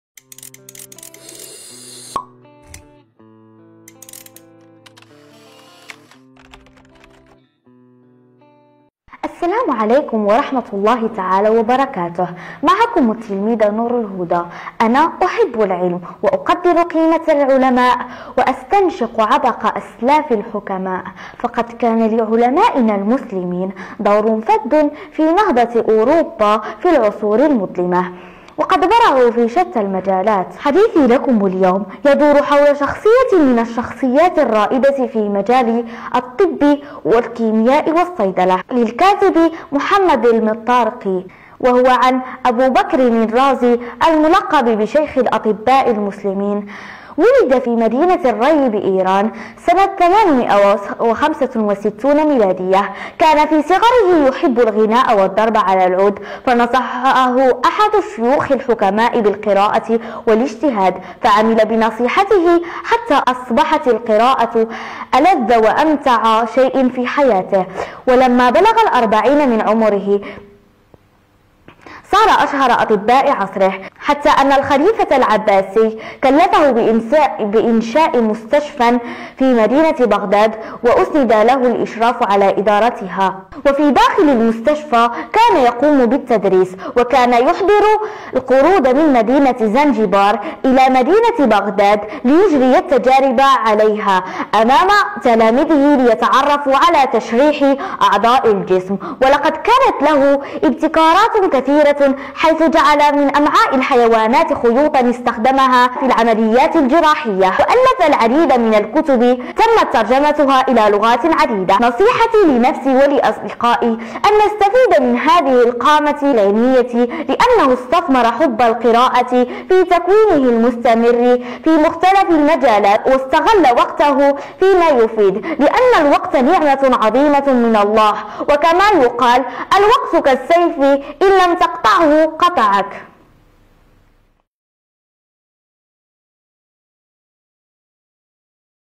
همسة قارئ 69: تقديم كتاب ابو بكر الرازي‎
تقديم الطفلة نور_الهدى_المفتوحي‎ لكتاب شخصيات لامعة